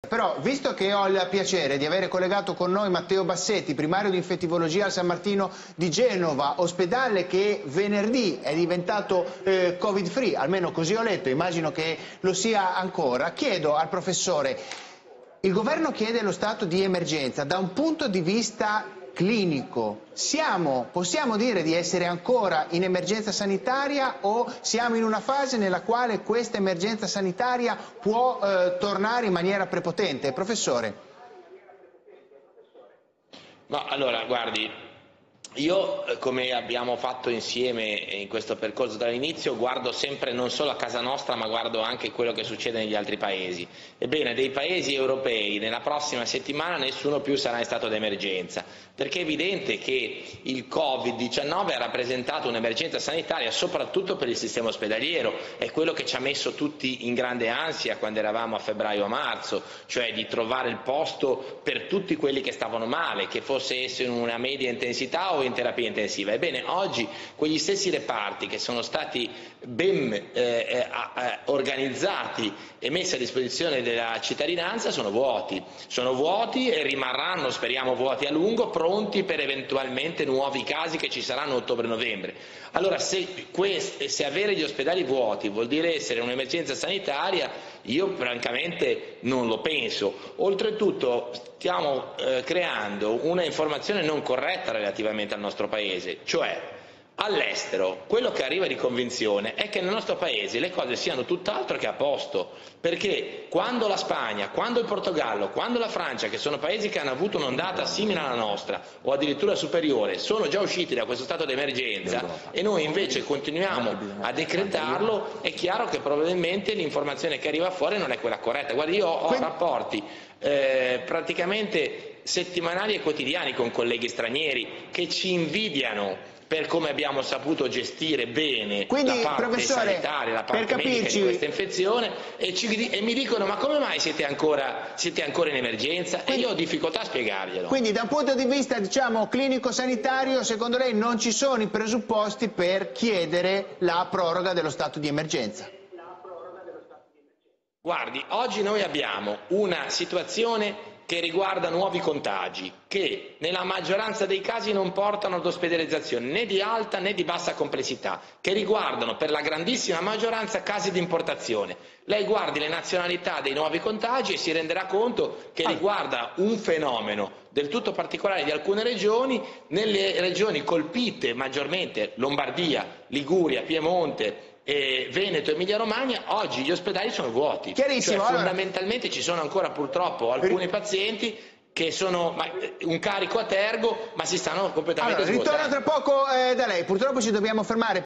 L'infettivologo Matteo Bassetti: "Dalla prossima settimana nessuno dei Paesi europei sarà in ...
Matteo Bassetti (infettivologo): "Il Covid 19 ha rappresentato un'emergenza sanitaria soprattutto per il sistema ospedaliero, oggi quegli stessi reparti soo vuoti e speriamo rimarranno vuoti a lungo. Dall'estero i colleghi mi chiedono come mai siamo ancora in stato d'emergenza e io ho difficoltà a dare una spiegazione".